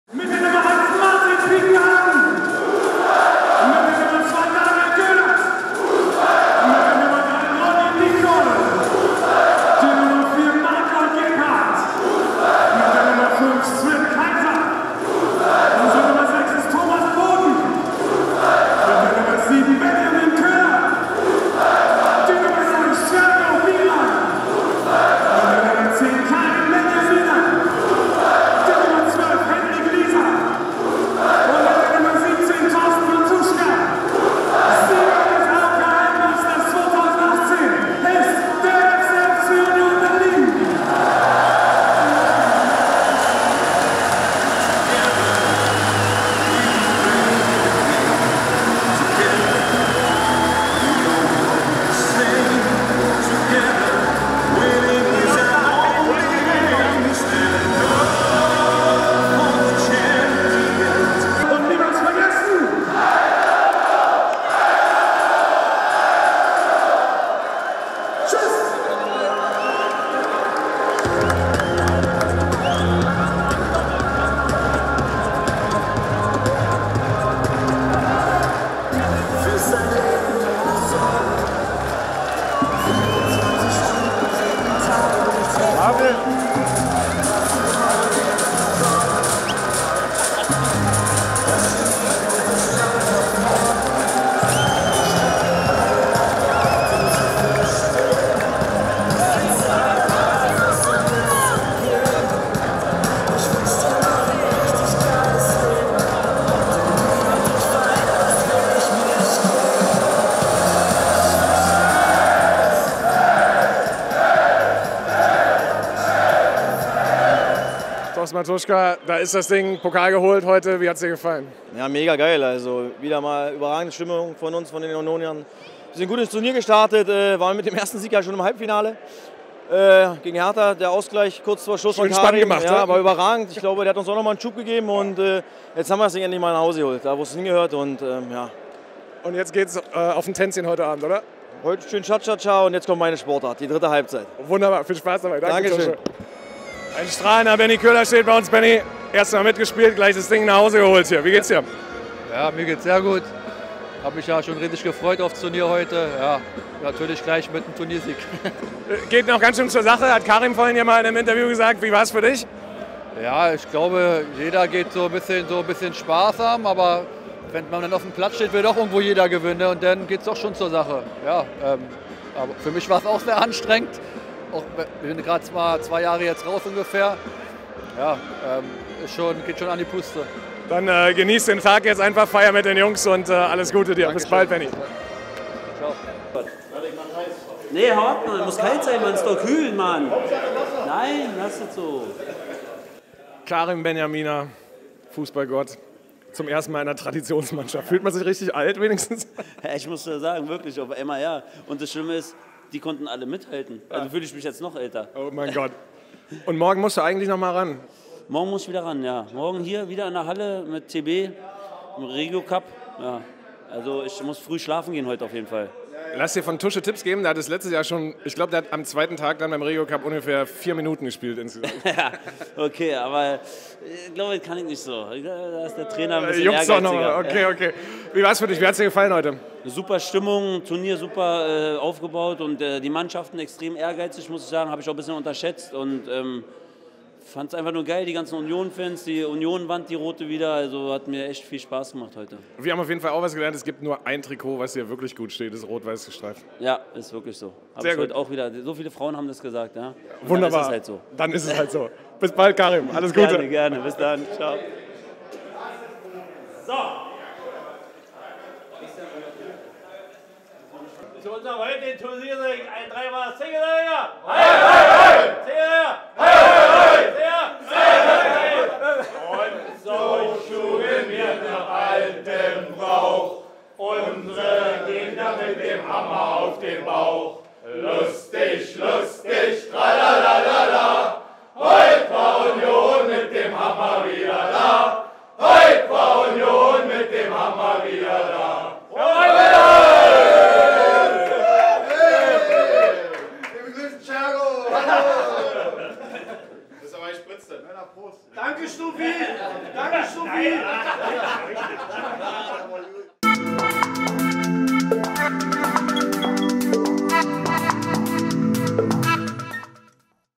Nach dem Sieg beim AOK Traditionsmasters 2018
Stimmung und Stimmen zum Sieg des 1. FC Union Berlin beim diesjährigen AOK Traditionsmasters am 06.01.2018 in der Max-Schmeling-Halle, Berlin.